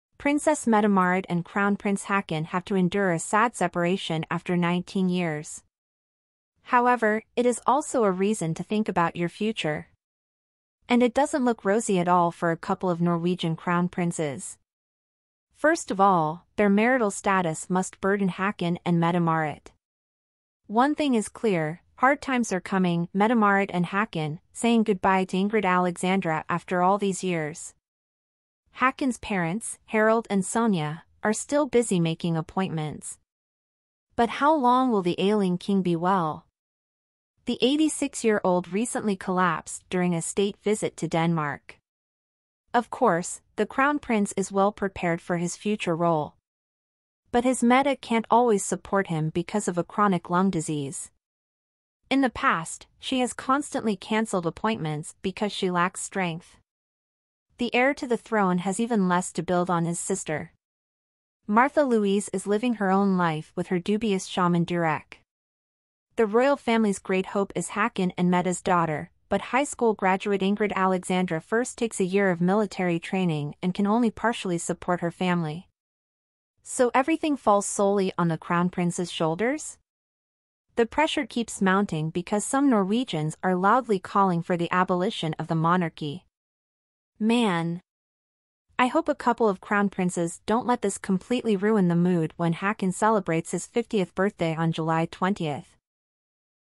Mette-Marit and Håkon: a really sad separation after 19 wonderful years!
Princess Mette-Marit and Crown Prince Håkon have to endure a sad separation after 19 years. However, it is also a reason to think about your future. And it doesn't look rosy at all for a couple of Norwegian Crown Princes. First of all, their marital status must burden Håkon and Mette-Marit. One thing is clear: hard times are coming.